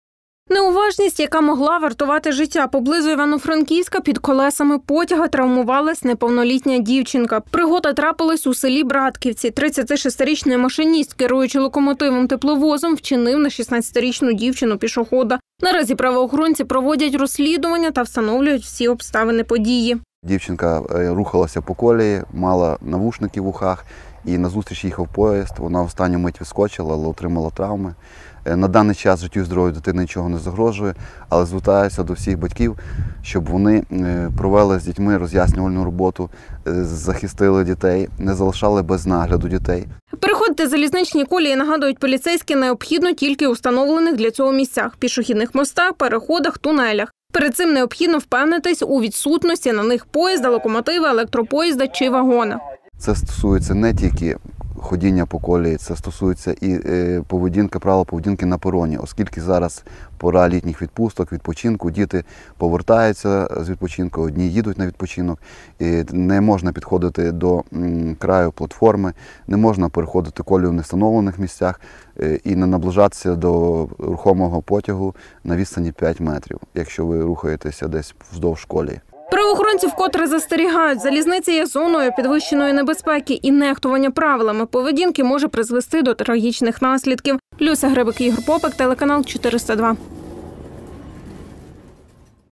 Поблизу Івано-Франківська під колесами потяга травмувалась неповнолітня дівчинка
Пригода трапилась у селі Братківці. Наразі правоохоронці проводять розслідування та встановлюють всі обставини події.